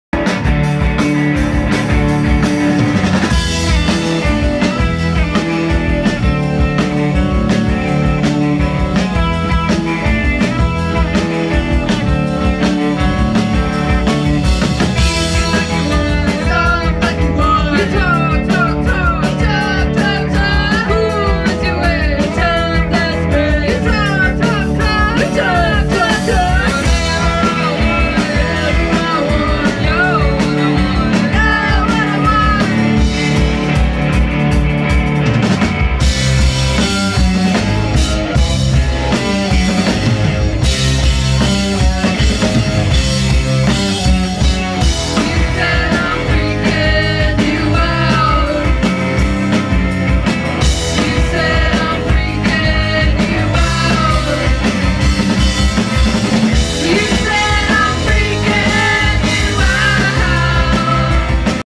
The Paranoyds perform synth punk at Barboza in Seattle 11/6/19
Synth punk show from L.A. band The Paranoyds. This was recorded live at Barboza music venue in Seattle, WA by Seattle Synth